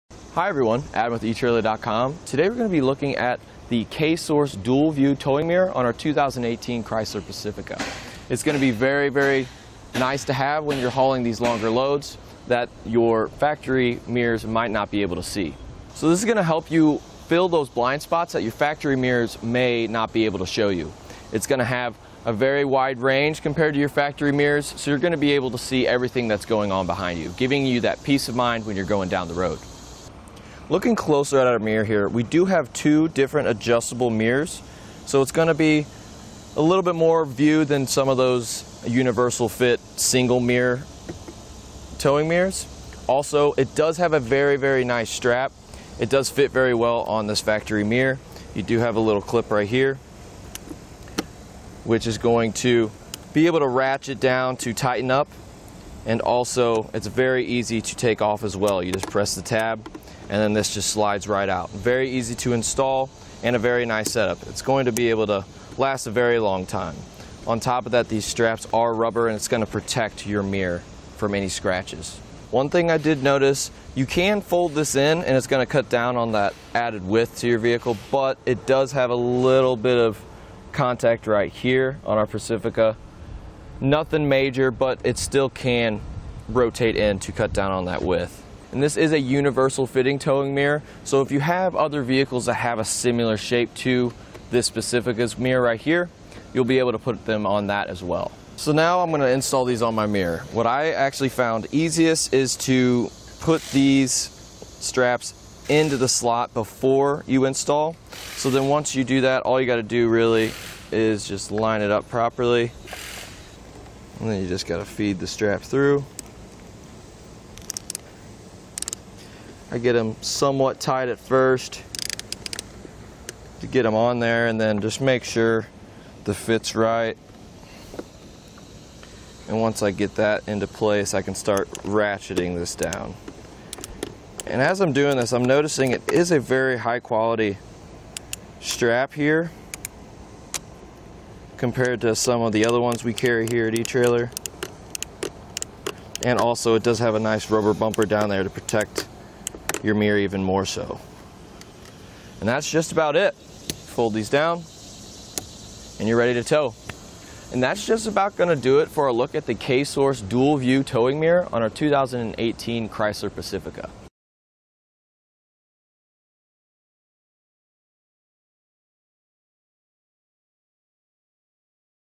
etrailer | Mount: K-Source Universal Dual Lens Towing Mirrors on a 2018 Chrysler Pacifica
Adam:               Hi, everyone. Adam with etrailer.com. Today we're going to be looking at the K-Source Dual-View Towing Mirror on our 2018 Chrysler Pacifica. It's going to be very, very nice to have when you're hauling these longer loads that your factory mirrors might not be able to see. So this is going to help you fill those blind spots that your factory mirrors may not be able to show you. It's going to have a very wide range compared to your factory mirrors.

So you're going to be able to see everything that's going on behind you, giving you that peace of mind when you're going down the road.Looking closer at our mirror here, we do have two different adjustable mirrors. So it's going to be a little bit more view than some of those universal-fit single mirror towing mirrors. Also, it does have a very, very nice strap. It does fit very well on this factory mirror. You do have a little clip right here, which is going to be able to ratchet down to tighten up and also it's very easy to take off as well.

You just press the tab, and then this just slides right out. Very easy to install and a very nice setup. It's going to be able to last a very long time. On top of that, these straps are rubber and it's going to protect your mirror from any scratches.One thing I did notice, you can fold this in and it's going to cut down on that added width to your vehicle, but it does have a little bit of contact right here on our Pacifica. Nothing major, but it still can rotate in to cut down on that width.

And this is a universal-fitting towing mirror. So if you have other vehicles that have a similar shape to this Pacifica's mirror right here, you'll be able to put them on that as well.Now I'm going to install these on my mirror. What I actually found easiest is to put these straps into the slot before you install. So then once you do that, all you got to do really is just line it up properly, and then you just got to feed the strap through. I get them somewhat tight at first, to get them on there, and then just make sure the fit's right.

And once I get that into place, I can start ratcheting this down. As I'm doing this, I'm noticing it is a very high-quality strap here compared to some of the other ones we carry here at etrailer. And, also, it does have a nice rubber bumper down there to protect your mirror even moreso. And that's just about it. Fold these down, and you're ready to tow.And that's just about going to do it for our look at the K-Source Dual-View Towing Mirror on our 2018 Chrysler Pacifica.